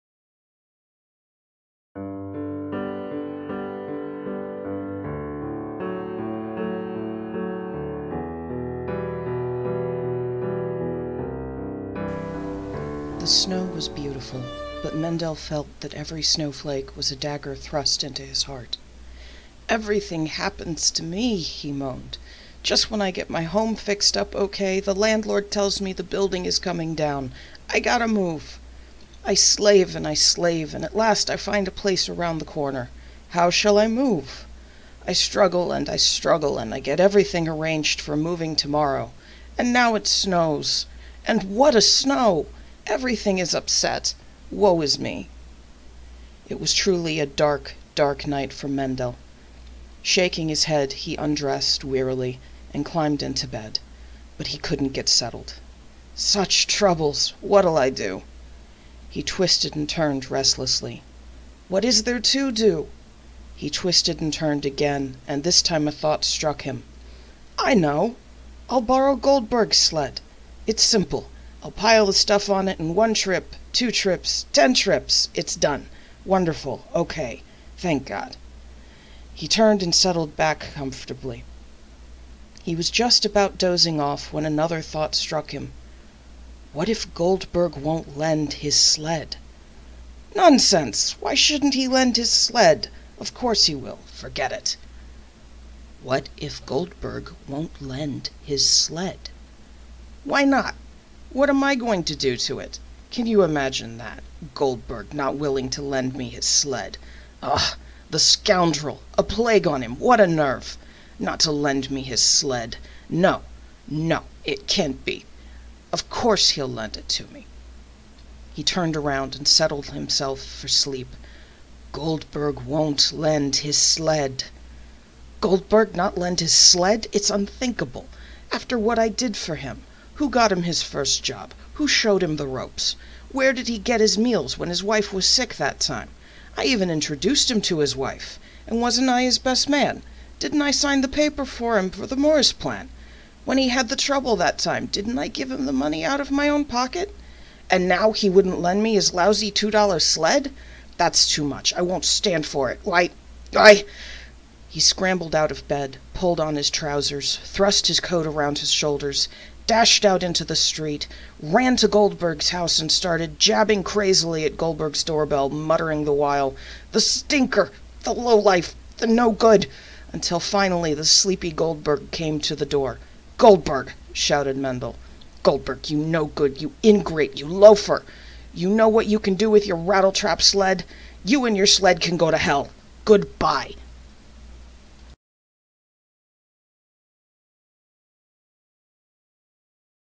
Story #5: The Sled Story, Jewish folktale read aloud
Jewish folktales for kids, read aloud for you
Story # 5: The Sled Story
I sympathized with Mendel while I read this story, but by the end I knew Goldberg must ber terribly confused.